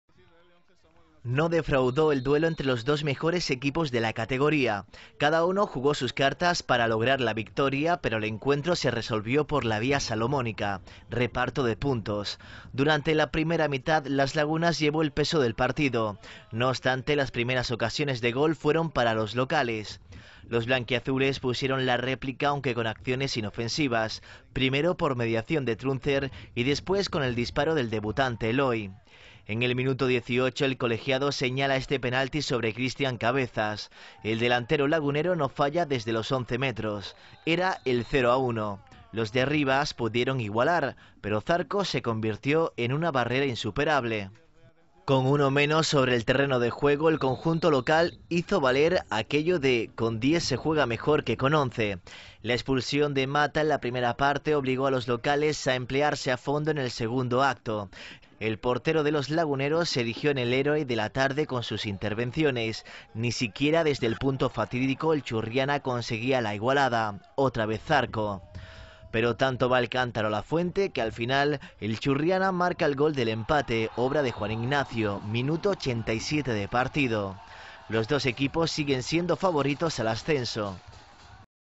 C.D. Churriana 1 - 1 A.D. Las Lagunas - Jornada 20
Encuentro de Regional Preferente de Málaga entre el Churriana y Las Lagunas. Jornada 20,  30.01.10